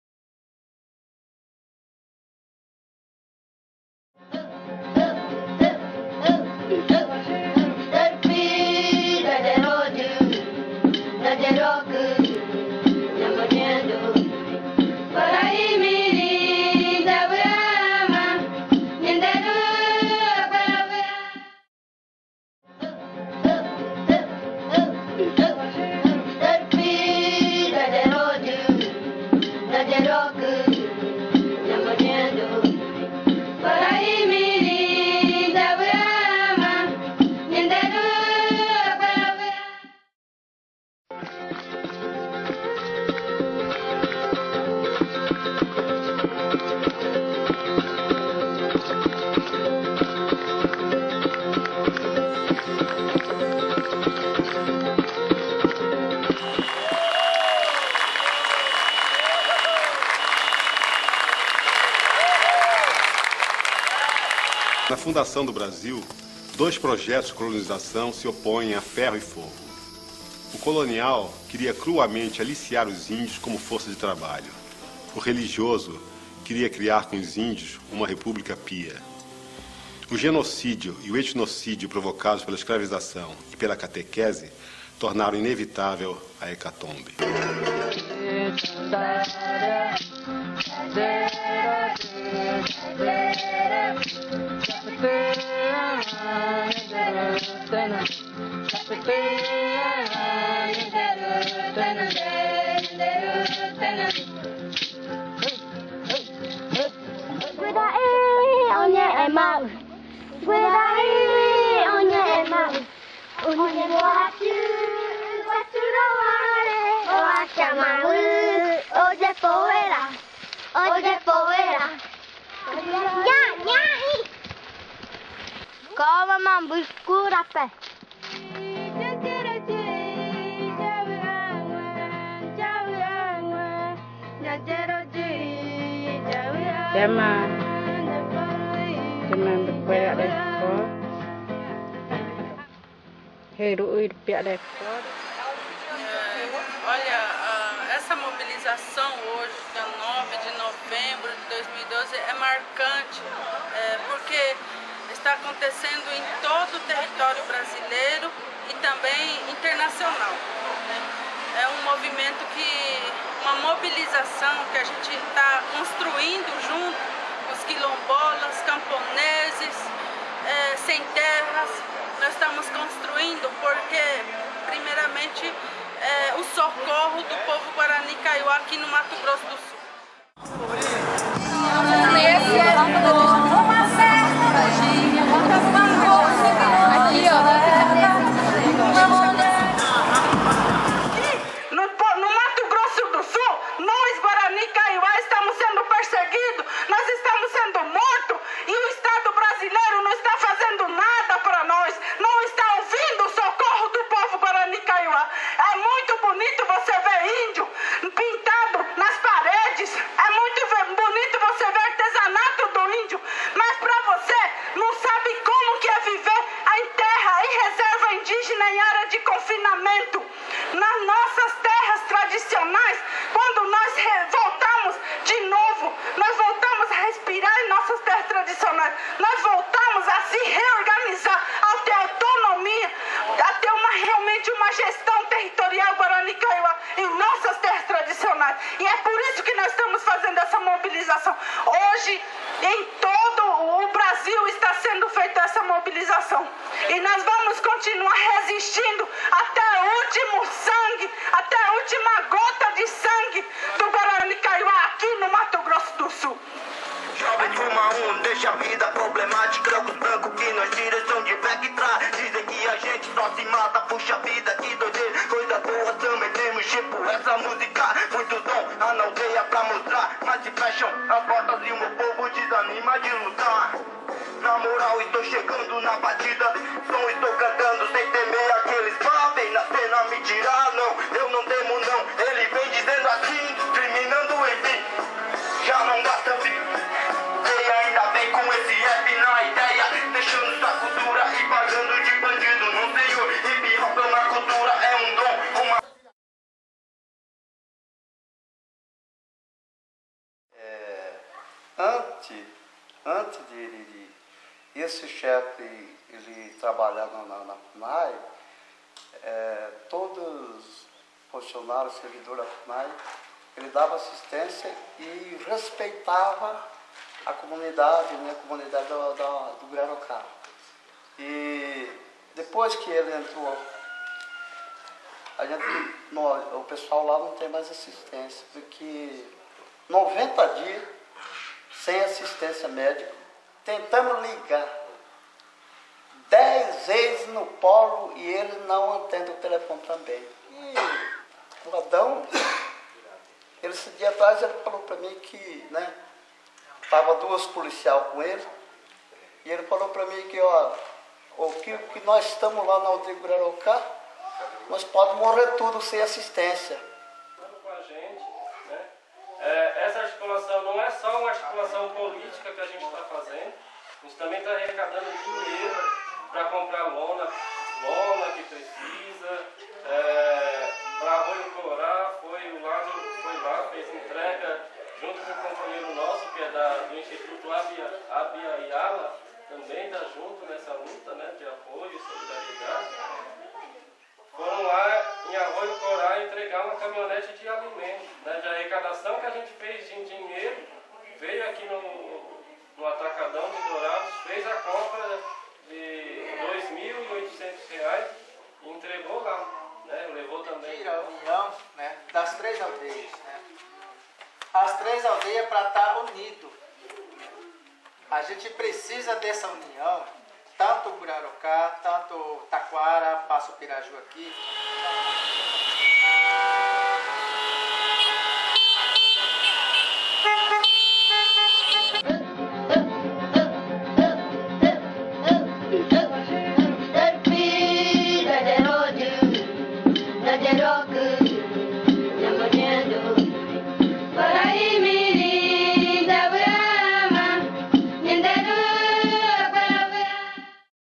Povos da floresta 2
O video enfoca a resistência do valoroso povo Guarani Kaiowá. No audiovisual, imagens da aldeia Passo Piraju e denúncias das lideranças da deste povo. Além disso,contém imagens  das intervenções das principais lideranças  que se manifestaram no ato do dia 9 de novembro em Dourados. Assistam ! compartilhem!